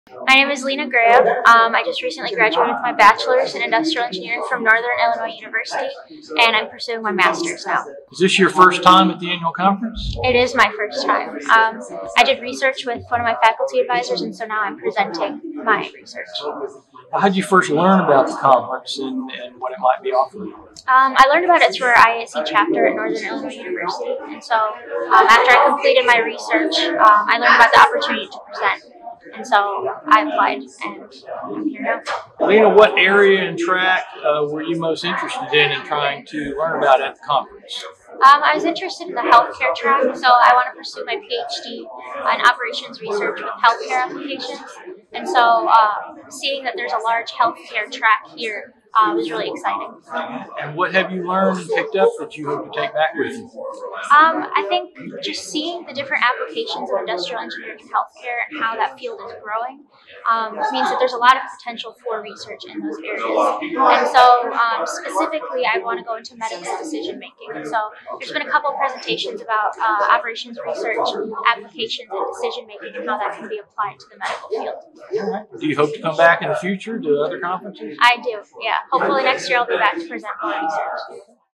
IISE Annual Conference & Expo 2019 - Leena Ghrayeb
ISE master’s student Leena Ghrayeb was wowed by the extensive healthcare systems engineering track during her first Annual Conference visit in 2019 in Orlando, Florida. Ghrayeb, who told ISE magazine Managing Editor Keith Albertson that she aims to pursue a Ph.D., said was impressed by several presentations about operations research in healthcare decision-making.